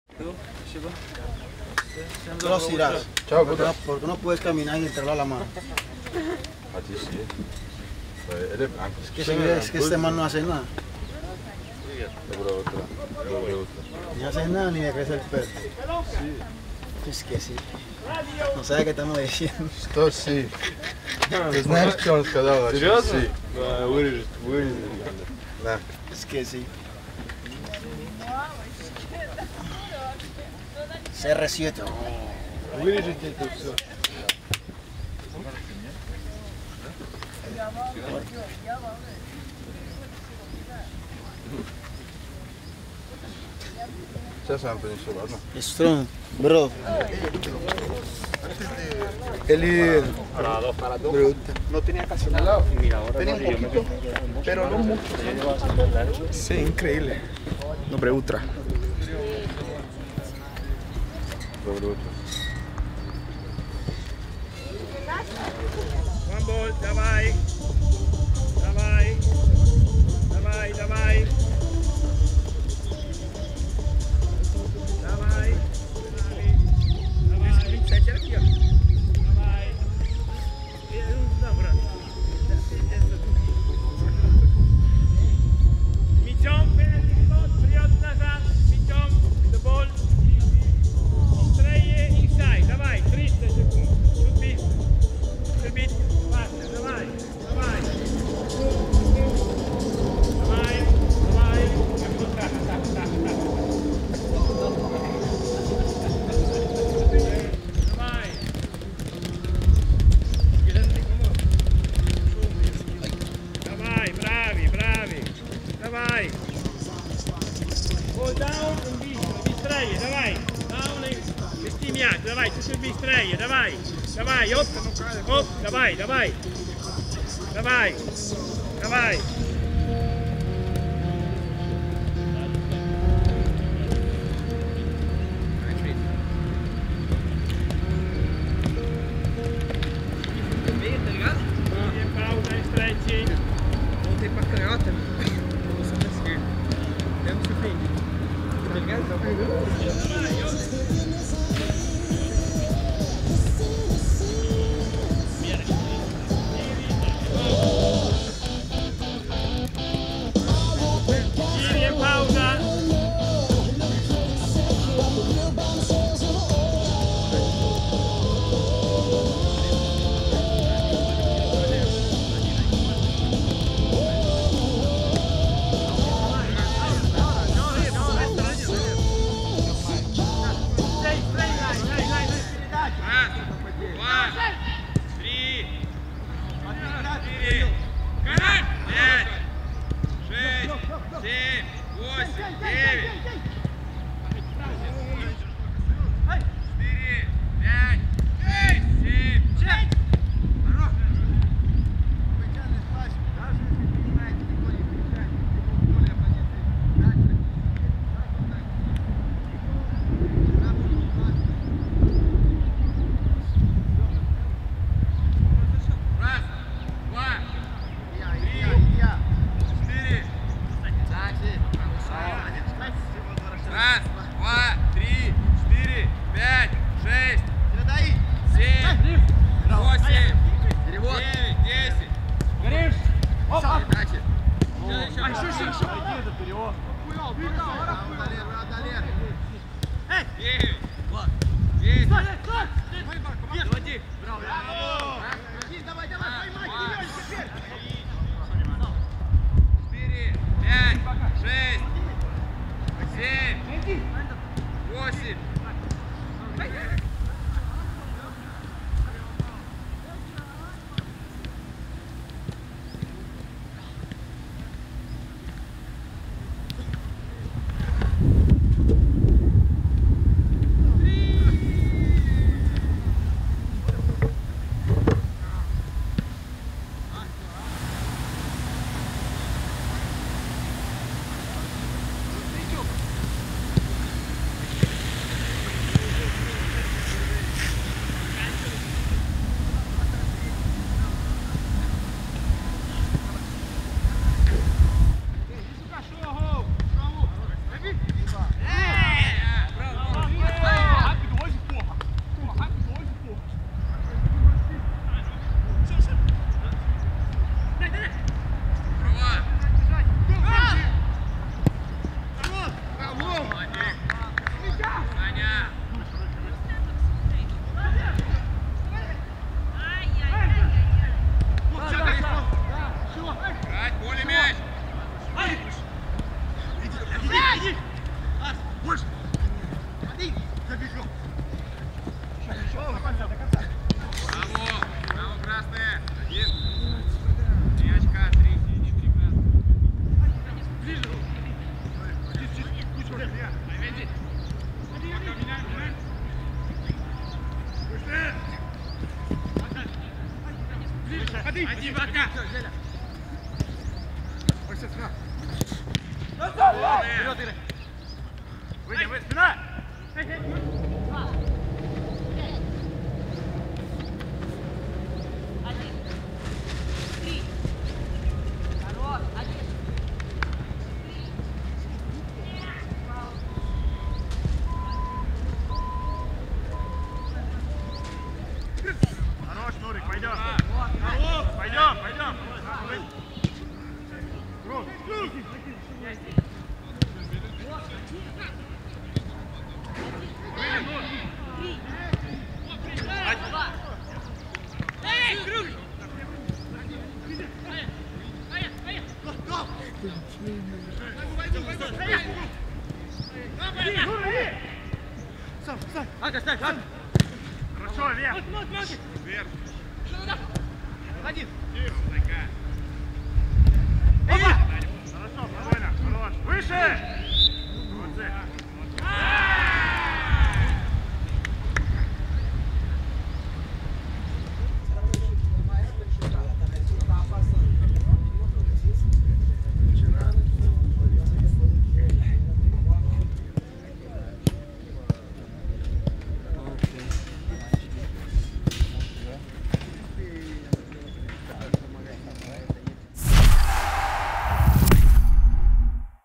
ДНЕВНИК СБОРОВ: видео, которое просили вырезать
На ваших экранах — восьмой день «Зенита» в Дубае!